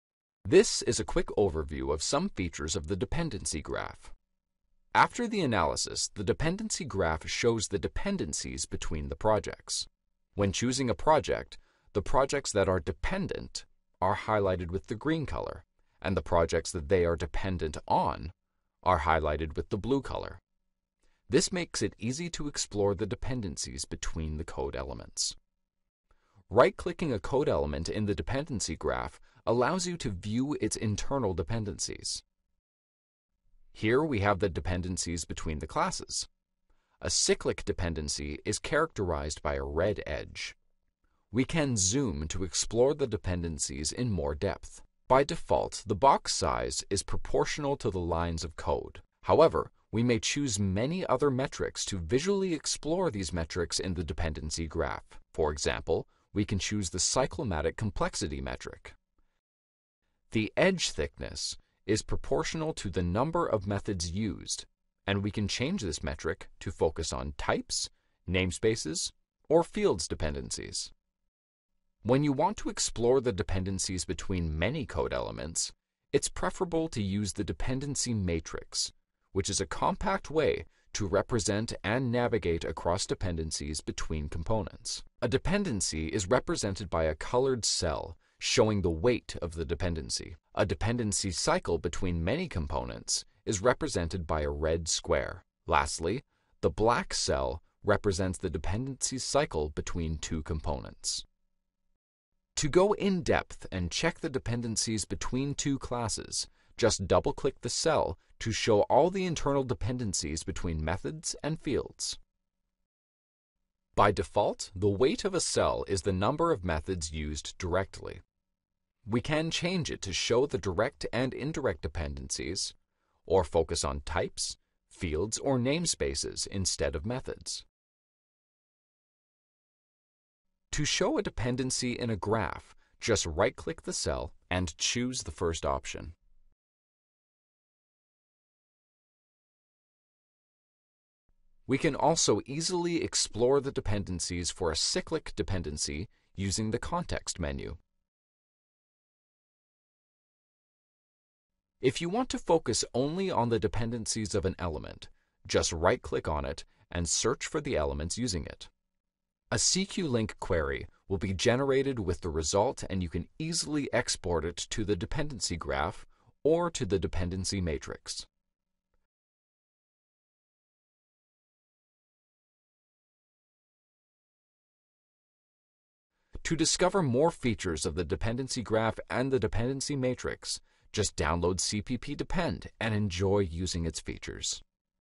CppDepend Graphs
CppDepend outputs several kinds of diagrams: Dependencies Graph, Dependencies Matrix, Abstracness vs. Instability, Treemap Metric, Tren Charts